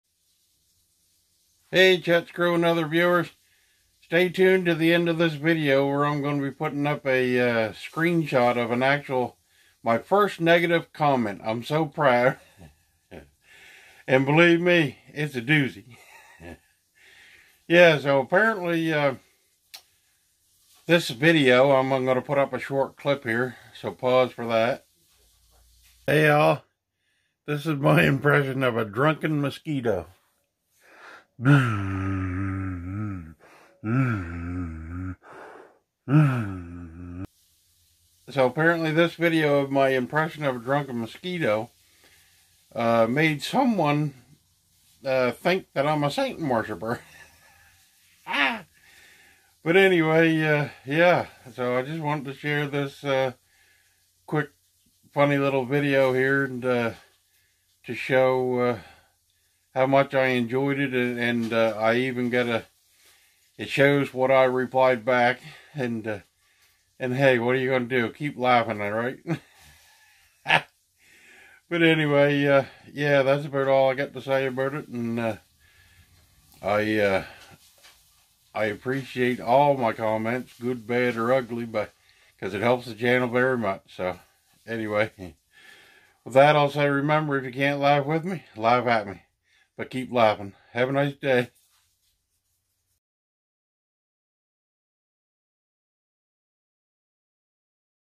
First negative comment: wait for the end, it's great!
Another funny short. My first negative comment, and man, it's a doozy. I added the actual post at the end; I've blurred the name and picture to protect their identity.